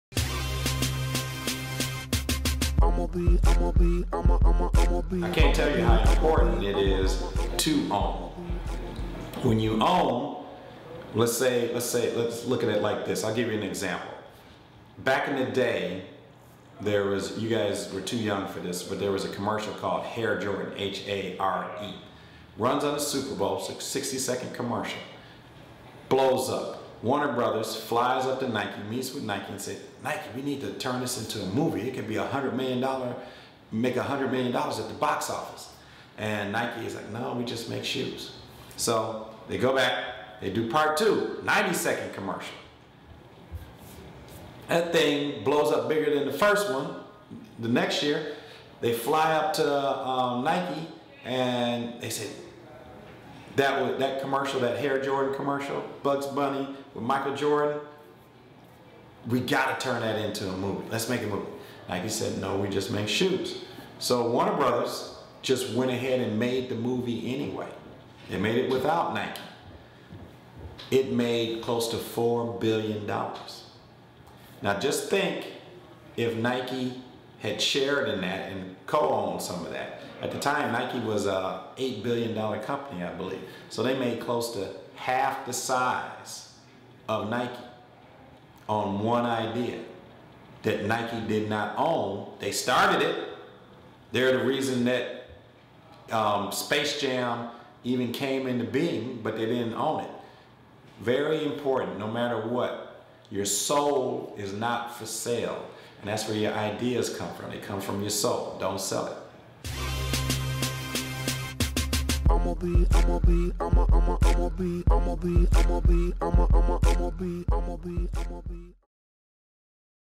JIMMY SMITH: OWN YOUR OWN DESTINY & FUTURE
Jimmy Smith of Amusement Park Entertainment teamed up with our jrCEOs™ as a mentor to talk about Entertainment and Advertising.

These videos are geared to inspire the young entrepreneur looking to learn about different types of jobs they can start.